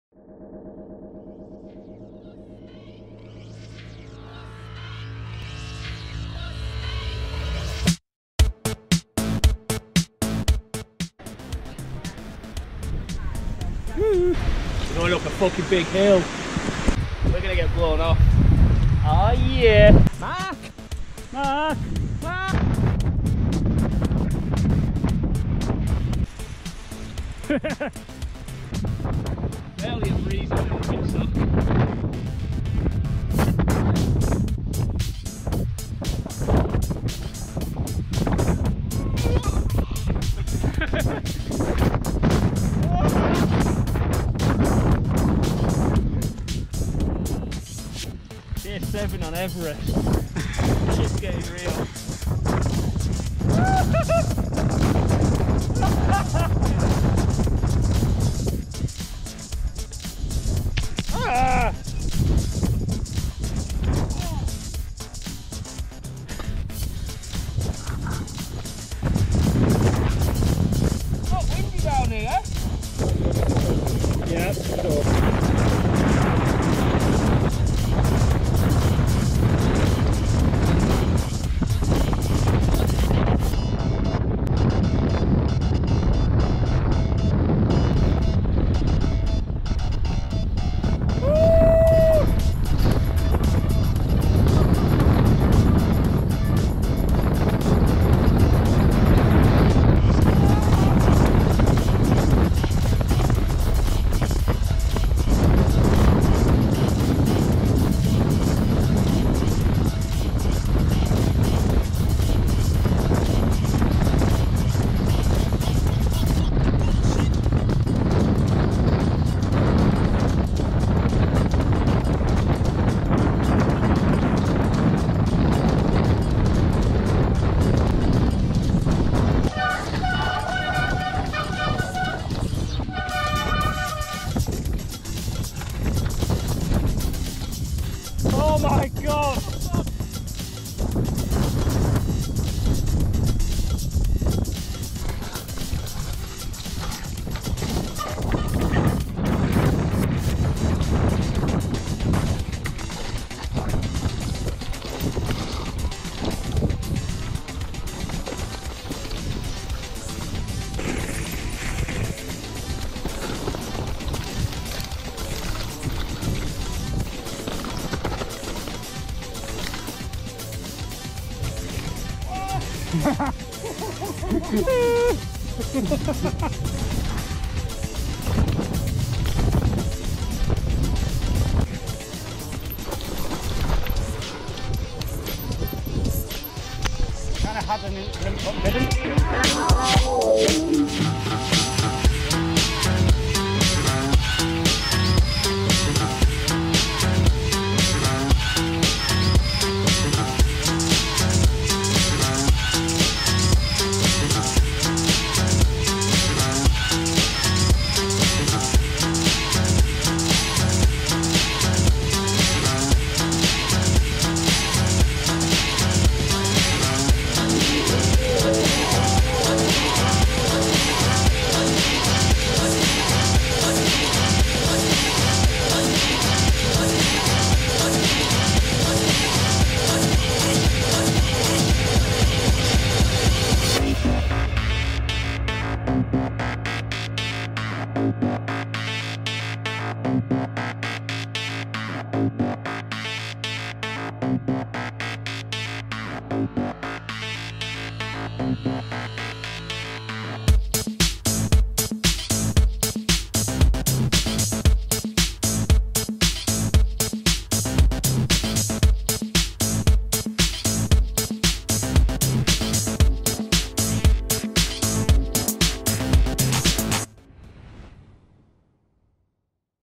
Grisedale Pike - A Beautiful Lake District MTB Ride
There’s nothing better than a great hike-a-bike up a fell, then riding down an amazing ridge with epic views. After all it’s what Lake District mountain biking is all about!

Let’s be fair, the wind was a little on the high side. Probably gusting a good 60mph+ on the ridge. We did have to get to ground and wrap our legs around our bikes a few times, which may have people saying we were idiots for venturing up there. Well, hey ho, now we know!

After an amazing burger in Keswick it’s was time for a bit of breezy pump track action. Be careful if you decide to ride a pump track after a big meal, it makes you feel a little sick . . . . . .

Sorry for the bad Language, we shall strive to not be as potty mouthed in the future, I'm not promising anything though . .